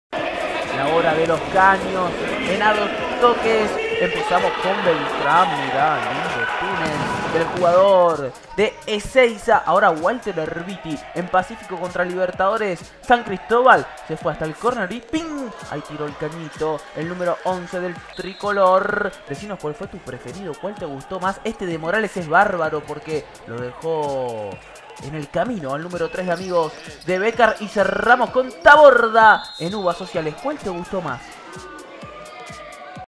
ElHabilidosoDelDia La segunda jornada de la Fecha1 nos dejó estos lindos lujos
ElHabilidosoDelDia La segunda jornada de la Fecha1 nos dejó estos lindos lujos 👌👏⚽👍👊📽️ ¿Cuál te gustó más? 🤗🤔
1- Joaquin Beltran de MunicipalidadDeEzeiza
2- Walter Erviti de Pacifico
3- Alejandro Morales de Necochea
4- Juan Taborda de UBASociales